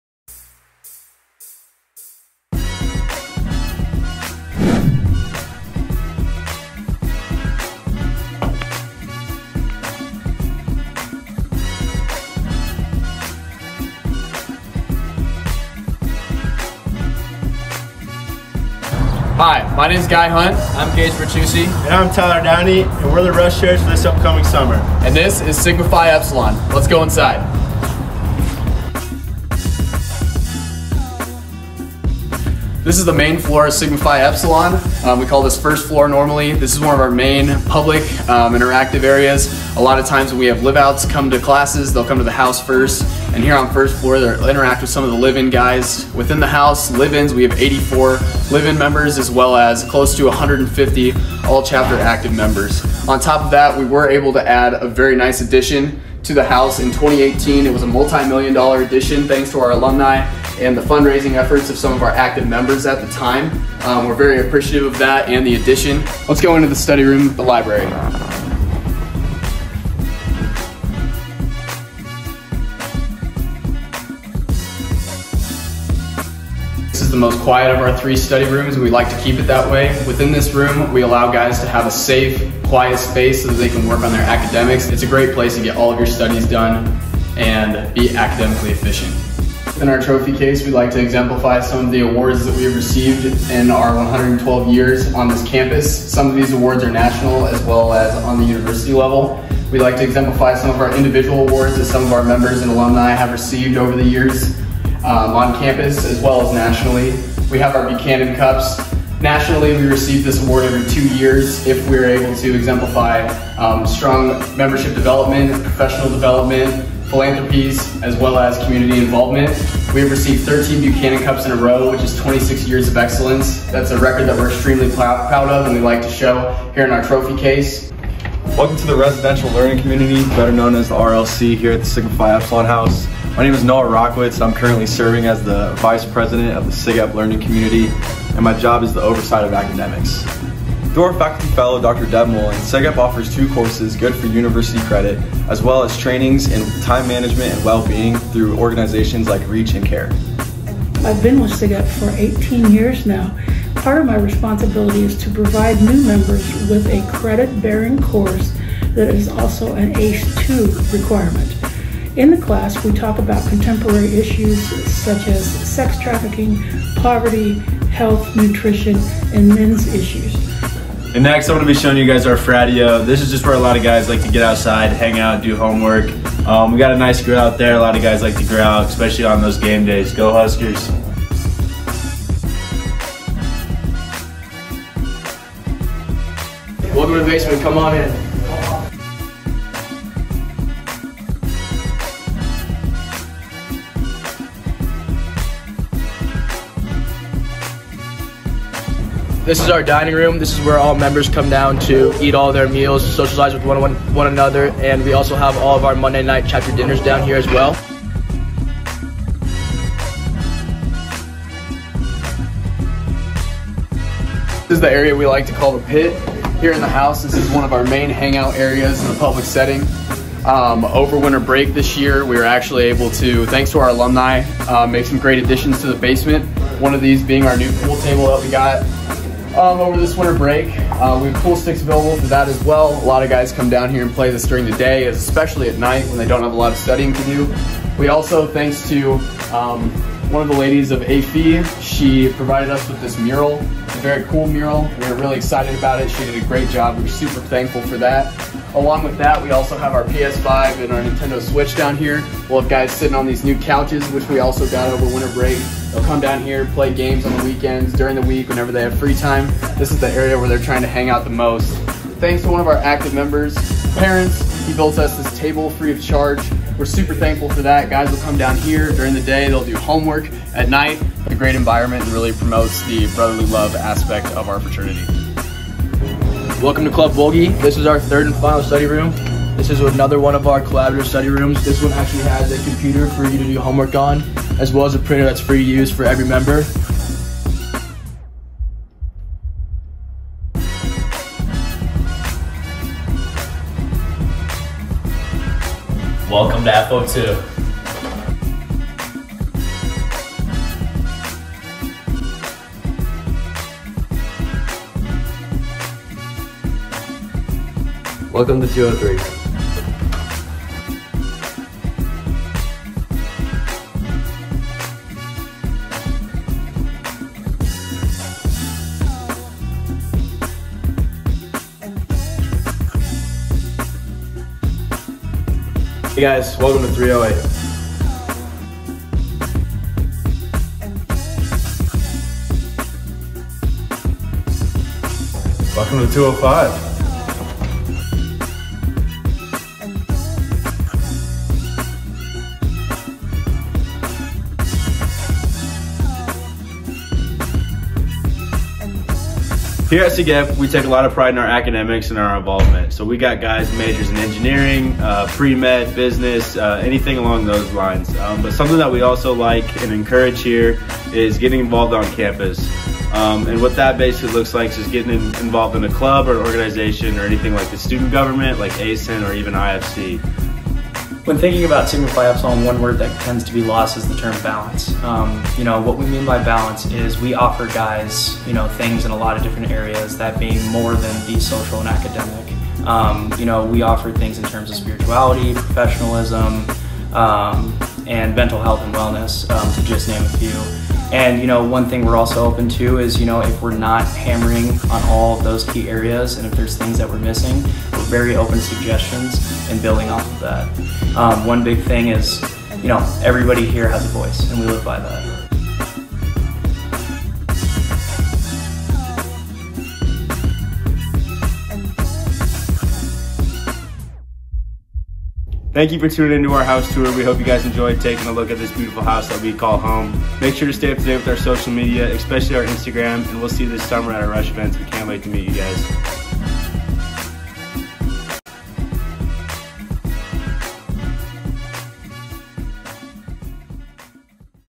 SigEp Nebraska Alpha 2023 House Tour
Come with us on a tour of the SigEp Nebraska Alpha Chapter House!
Look out for information about rush events coming this summer!